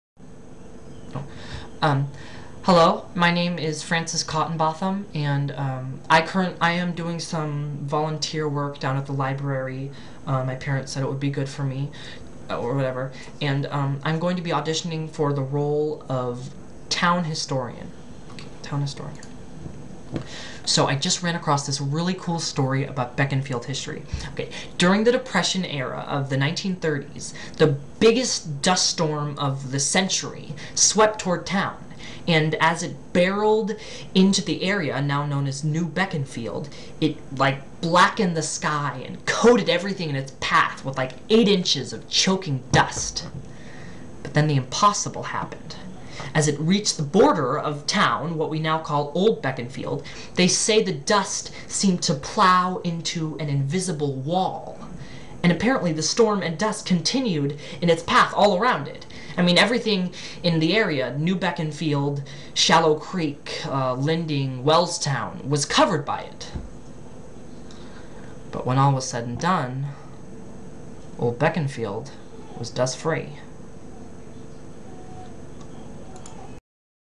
Ultimate Online Audition - Frances Cottinbothom Auditions for "Young Historian"
Round 1

Frances Cottinbothom, played by Drew Monson auditions for the role of Young Historian